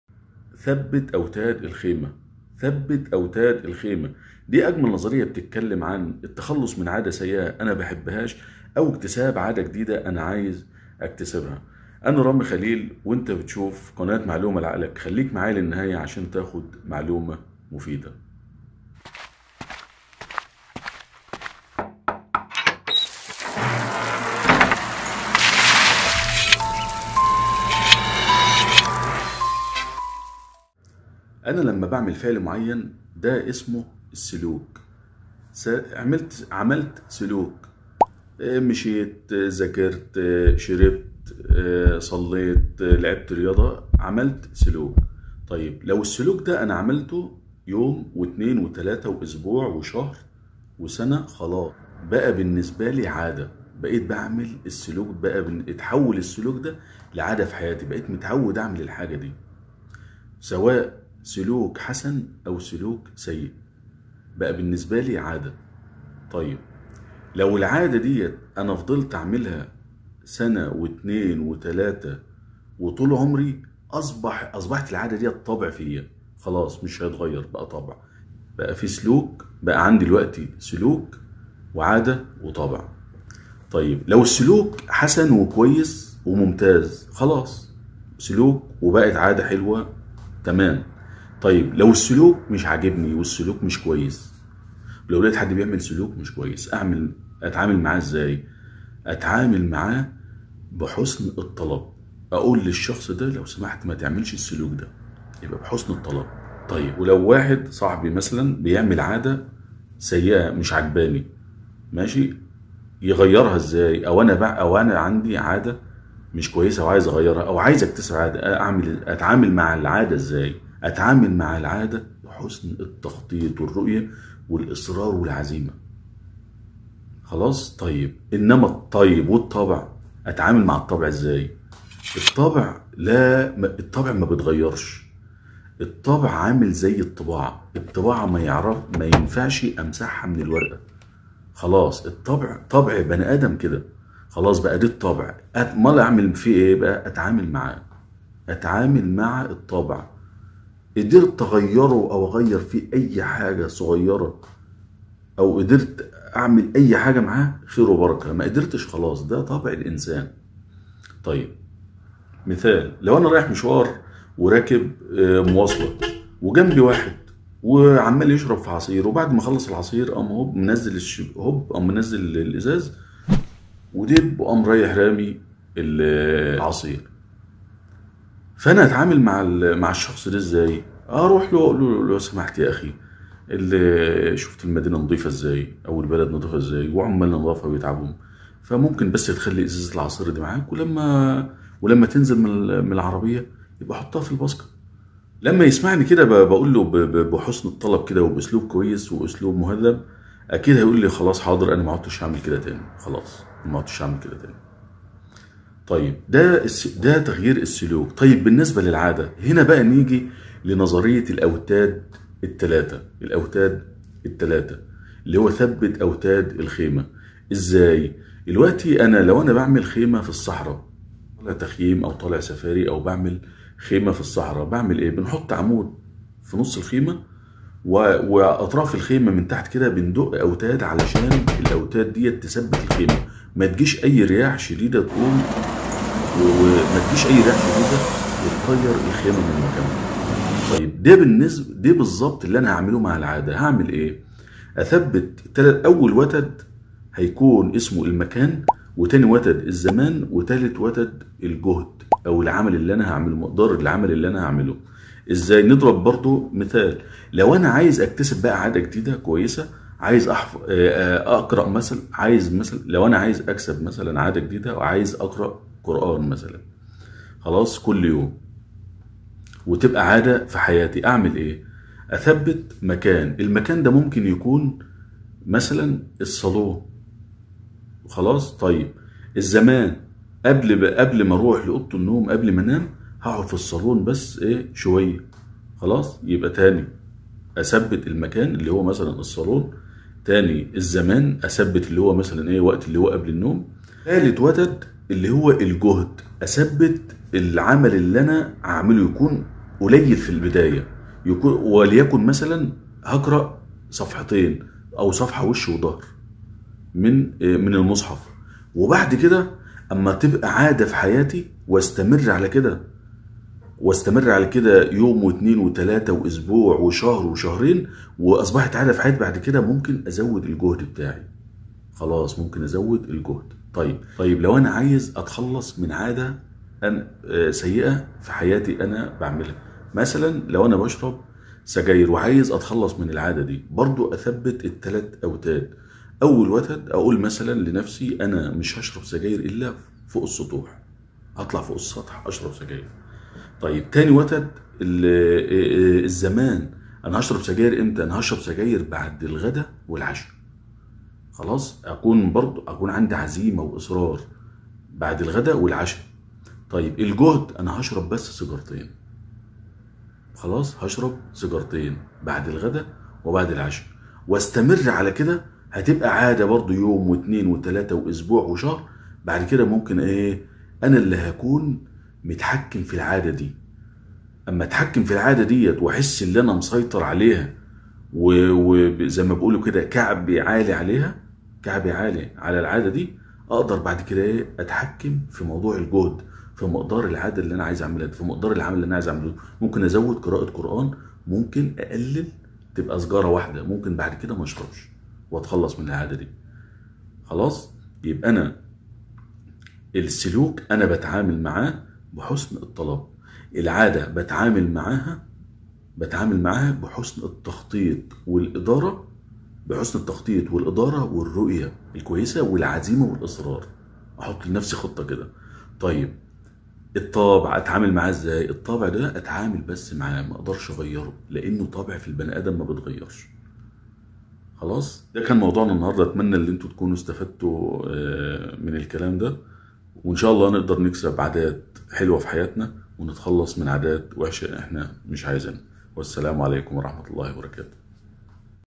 اوتاد الخيمة
اوتاد الخيمة افضل نظرية فى محاولة اكتساب اى عادة جديدة
او التخلص من عادة سيئة تؤرق الشخص
الفرق بين
السلوك
العادة
الطبع
لابد لكل شخص يعرف الفرق بينهم ويجلس مع نفسه ويراقب  تصرفاته
كيف اكتسب عاده جديده
كيف اتخلص من عادة سيئة
المكان
الوقت
الجهد
هى الاوتاد المهمة الى جانب النية الحسنه والإصرار والعزيمة
مصادر معلومة لعقلك:-
كتاب فن الكلام للدكتور ايهاب فكرى